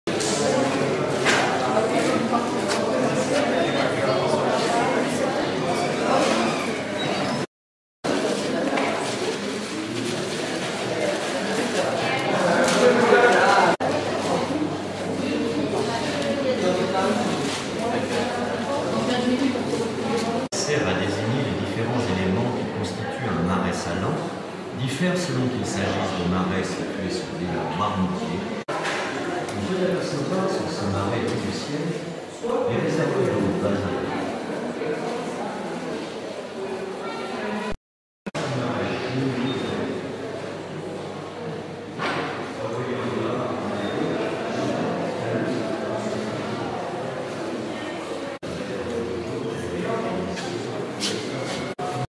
Gurande_ (En) Salt museum,_(Id) Museum Garam
(En) Salt museum, (Id) Museum Garam

Kunjungan  CROUS de Rennes ke museum garam Guérande